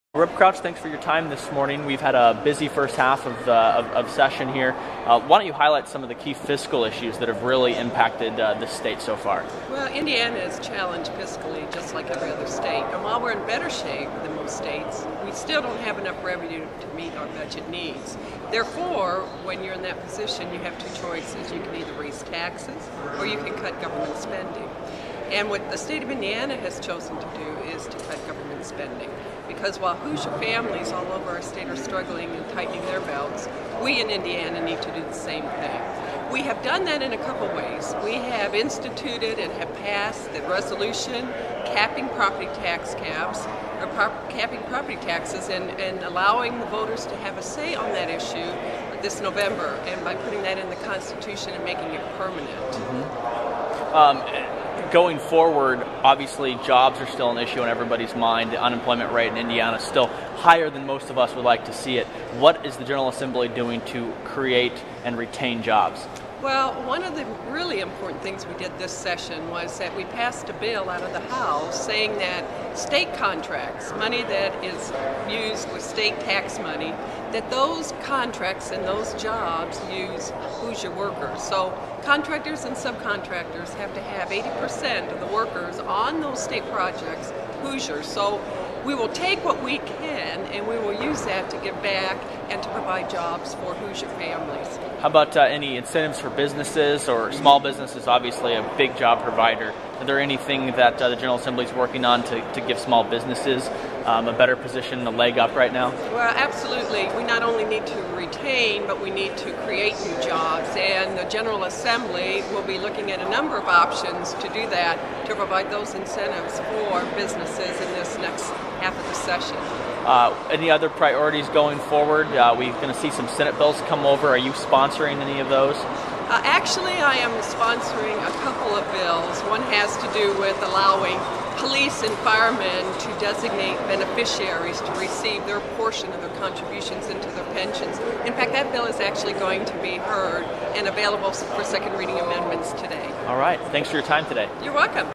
Rep. Crouch Discusses Second Half Legislation
Rep. Suzanne Crouch (R-Evansville) discusses issues surrounding the second half of the 2010 legislative session. (February 11, 2010)